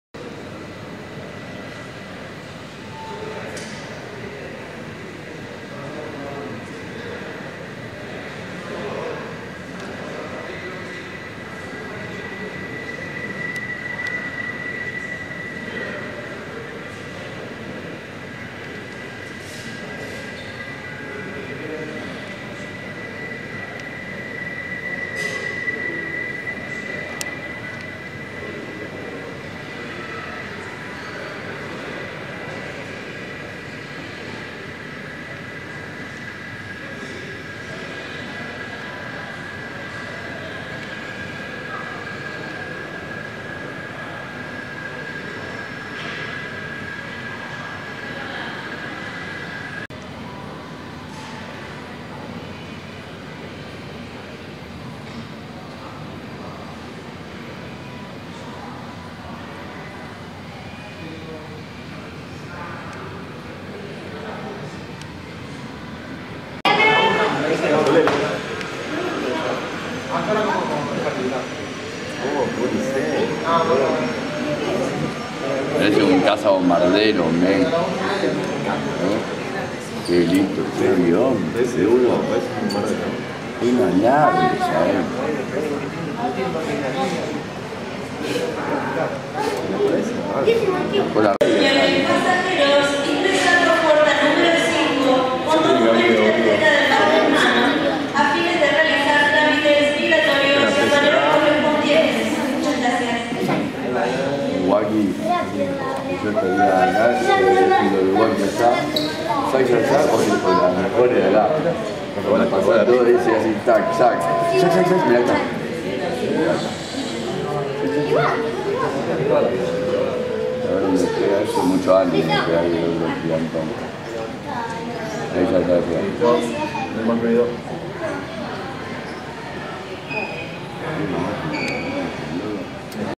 Bombardier Learjet 60 LV-CZX de Baires Fly S.A. en Rosario 09-07-2013
Video tomado el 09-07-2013, desde la terminal del aeropuerto de Rosario, espectacular aterrizaje de este avión pero por las condiciones climáticas no sale en el video. Me llamo la atención el espectacular esquema de pintura y la amabilidad de los pilotos de Baires Fly S.A. ayudando a la señora con el paraguas y antes de despegar movieron el avión para que los pasajeros se mojen lo menos posible